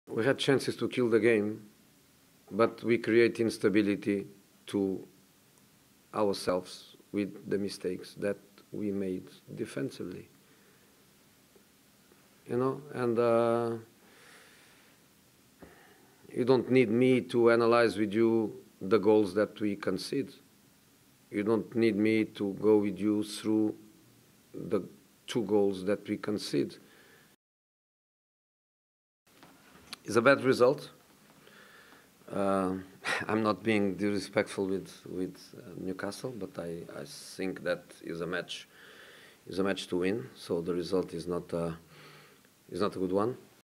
'We had chances to kill the game' -Tottenham boss Mourinho on late Newcastle equalizer 热刺客平纽卡斯尔 穆里尼奥
Tottenham missed the chance to move into the top four of the Premier League following a 2-2 draw away to Newcastle on Sunday who equalised late through Joe Willock.
That result leaves Spurs level in fifth place with Liverpool and two points adrift of fourth-placed Chelsea with eight games remaining.